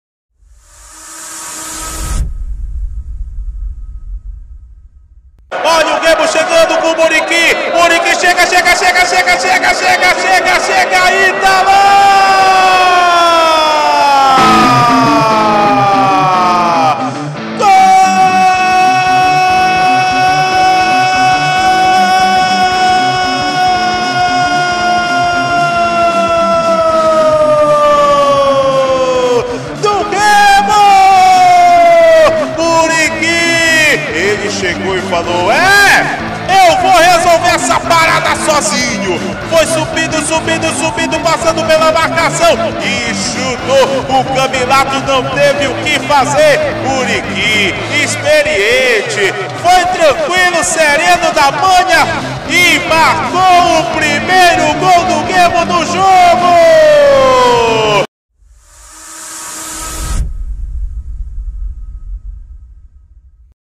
REACT & NARRAÇÃO: VITÓRIA-ES 0 X 1 REMO - NARRAÇÃO
Prepara teu churrasquinho de gato e a tua cerveja gelada que já já vai começar mais um jogo emocionante com aquela narração que você só encontra na RS Live Sports!

Bora pro jogo que a chinela vai cantar!

Me siga no instagram: @rsantoscomunica